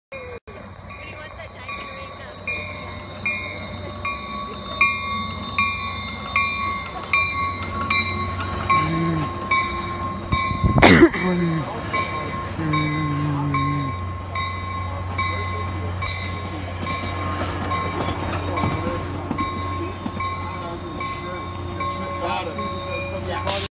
Ace 3101 5:30 is arriving into vasco station in Livermore
Ace 3101 leads ace #6 into vasco station in Livermore